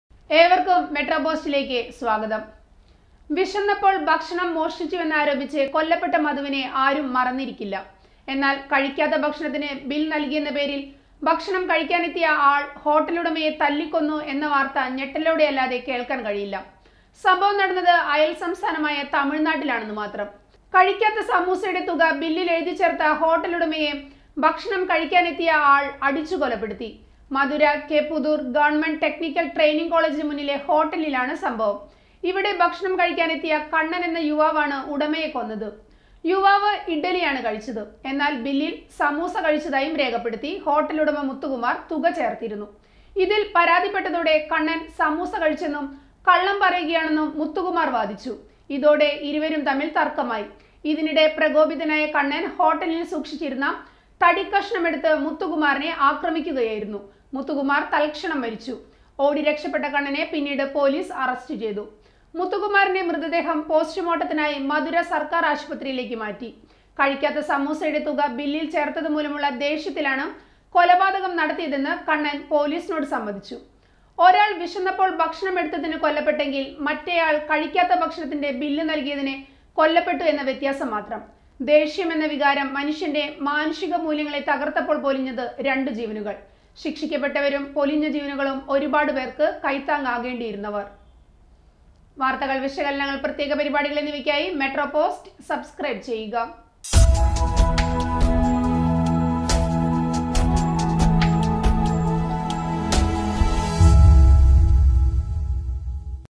കഴിക്കാത്ത ഭക്ഷണത്തിന് ബില്ല് , ഹോട്ടലുടമയെ അടിച്ചു കൊന്നു | METRO POST
കഴിക്കാത്ത ഭക്ഷണത്തിന്  ബില്ല് , ഹോട്ടലുടമയെ അടിച്ചു കൊന്നു

വിശന്നപ്പോൾ ഭക്ഷണം മോഷ്ടിച്ചുവെന്നാരോപിച്ചു കൊല്ലപെട്ട  മധുവിനെ ആരും മറന്നിരിക്കില്ല. എന്നാൽ കഴിക്കാത്ത ഭക്ഷണത്തിനു ബിൽ നൽകിയെന്ന പേരിൽ ഭക്ഷണം കഴിക്കാനെത്തിയ ആൾ ഹോട്ടലുടമയെ തല്ലി  കൊന്നു എന്ന വാർത്ത ഞെട്ടലയോടെ അല്ലാതെ കേൾക്കാൻ കഴിയില്ല.സംഭവം നടന്നത് അയാൾ സംസ്ഥാനമായ തമിഴ്നാട്ടിലാണെന്നു മാത്രം.

കഴിക്കാത്ത സമൂസയുടെ തുക ബില്ലിൽ എഴുതിച്ചേർത്ത ഹോട്ടലുടമയെ ഭക്ഷണം കഴിക്കാനെത്തിയ ആൾ അടിച്ചു കൊലപ്പെടുത്തി.മധുര കെ പുദൂർ ഗവ.ടെക്‌നിക്കൽ ട്രെയിനിങ് കോളജിന് മുന്നിലെ ഹോട്ടലിലാണു സംഭവം. ഇവിടെ ഭക്ഷണം കഴിക്കാനെത്തിയ കണ്ണൻ എന്ന യുവാവ് ആണ് ഉടമയെ കൊന്നത്.

യുവാവ്   ഇഡ്ഡലിയാണു കഴിച്ചത്. എന്നാൽ ബില്ലിൽ സമൂസ കഴിച്ചതായും രേഖപ്പെടുത്തി ഹോട്ടലുടമ മുത്തുകുമാർ തുക ചേർത്തിരുന്നു. ഇതിൽ പരാതിപ്പെട്ടതോടെ കണ്ണൻ സമൂസ കഴിച്ചെന്നും കള്ളം പറയുകയാണെന്നും മുത്തുകുമാർ വാദിച്ചു. ഇതോടെ ഇരുവരും തമ്മിൽ തർക്കമായി. ഇതിനിടെ പ്രകോപിതനായ കണ്ണൻ ഹോട്ടലിൽ സൂക്ഷിച്ചിരുന്ന തടിക്കഷണമെടുത്ത് മുത്തുകുമാറിനെ ആക്രമിക്കുകയായിരുന്നു.മുത്തുകുമാർ തൽക്ഷണം മരിച്ചു. ഓടി രക്ഷപ്പെട്ട കണ്ണനെ പിന്നീട് പൊലീസ് അറസ്റ്റ് ചെയ്തു.

മുത്തുകുമാറിന്റെ മൃതദേഹം പോസ്റ്റ്മോർട്ടത്തിനായി മധുര സർക്കാർ ആശുപത്രിയിലേക്കു മാറ്റി. കഴിക്കാത്ത സമൂസയുടെ തുക ബില്ലിൽ ചേർത്തതു മൂലമുള്ള ദേഷ്യത്തിലാണു കൊലപാതകം നടത്തിയതെന്നു കണ്ണൻ പൊലീസിനോടു സമ്മതിച്ചു.

ഒരാൾ വിശന്നപ്പോൾ ഭക്ഷണം എടുത്തതിനു കൊല്ലപ്പെട്ടെങ്കിൽ മറ്റേ ആൾ കഴിക്കാത്ത ഭക്ഷണത്തിന്റെ ബില് നൽകിയതിന് കൊല്ലപ്പെട്ടു എന്ന വ്യത്യാസം മാത്രം. ദേഷ്യം എന്ന വികാരം മനുഷ്യന്റെ മാനുഷിക മൂല്യങ്ങളെ തകർത്തപ്പോൾ പൊലിഞ്ഞതു രണ്ടു ജീവനുകൾ. ശിക്ഷിക്കപെട്ടവരും പൊലിഞ്ഞ ജീവനുകളും ഒരുപാടു പേർക്ക് കൈത്താങ്ങാകേണ്ടിയിരുന്നവർ.